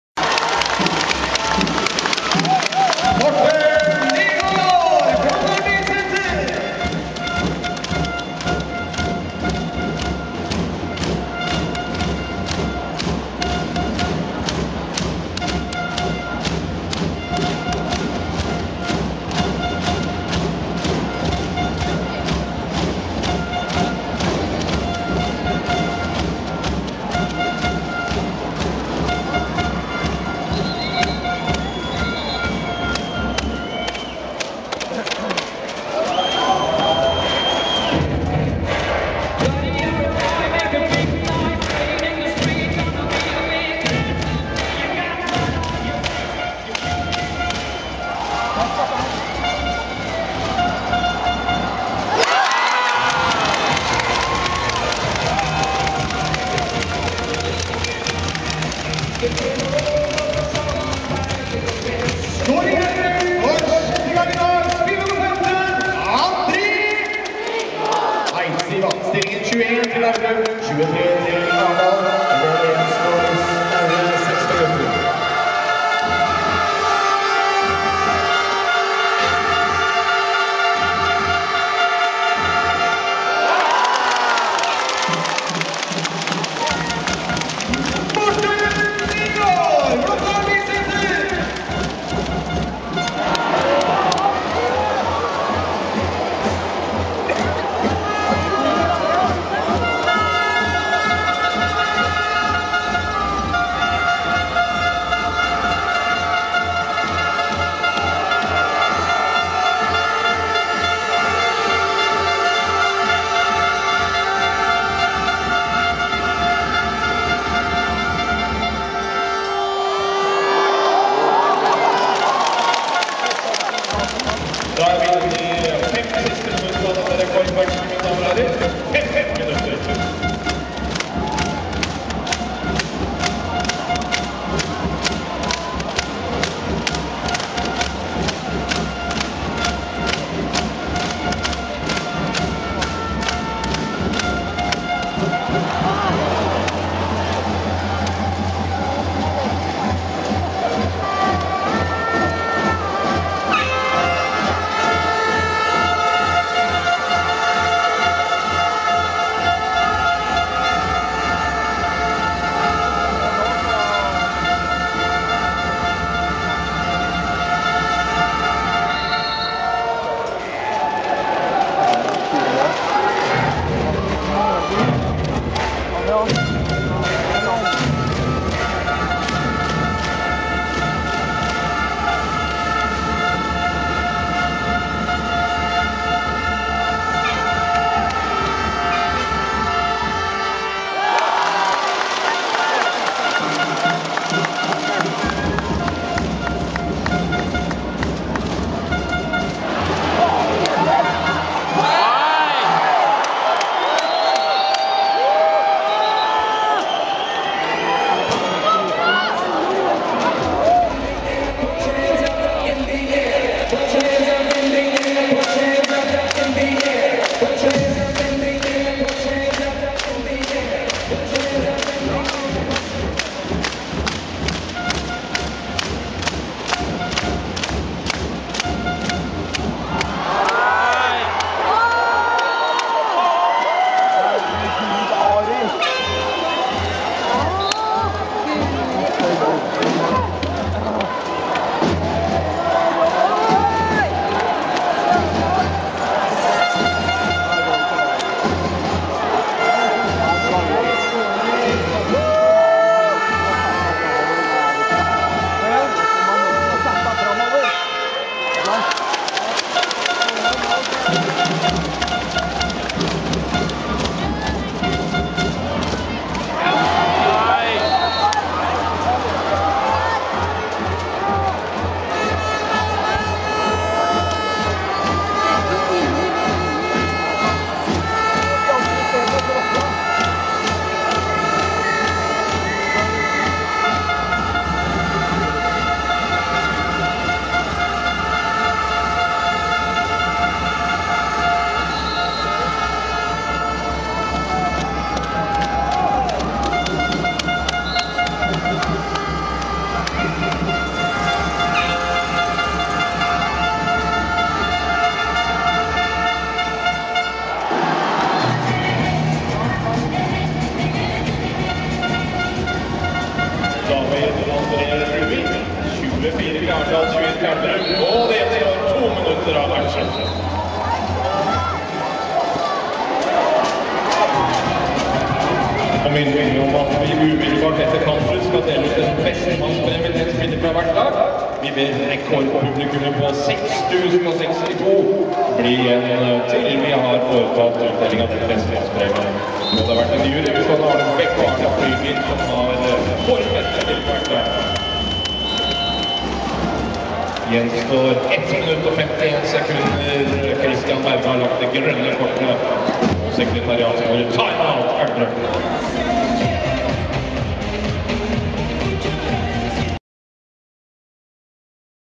Elverum - ØIF Arendal, REDIGERT versjon_Del 1. Postenligaen 27.10.12. Hamar.
REDIGERT versjon (av YouTube) av Elverum - ØIF Arendal. Postenligaen, Hamar OL-amfi, 27.10.12. Foto/video: Ole Jonny Trangsrud  Rosa Sløyfe-kamp. - Det stod 22-25 og det var igjen 1 minutt og 7 sekunder. Og det ble 25-25 (se del 3).